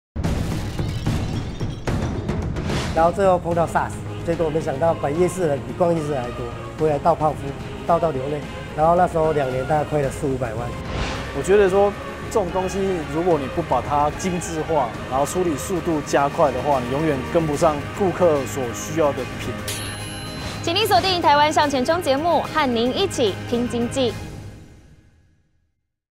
2019/11/09精彩預告
翻轉負債 主廚靠蛋糕代工重生

真的創業不是那麼簡單
也是跌跌撞撞
那時候兩年大概虧了四五百萬
碰到SARS
沒想到擺夜市的人
比逛夜市的人還多
回來倒泡芙 倒到流淚
因為生意真的非常差

白蝦網路團購夯 年營收上看千萬

老人家的一個想法
就是覺得說好我保守一點
我覺得說 這種東西你如果
不把它給那個精緻化
然後處理速度加快的話
你永遠跟不上就是
顧客所需要的品質這樣子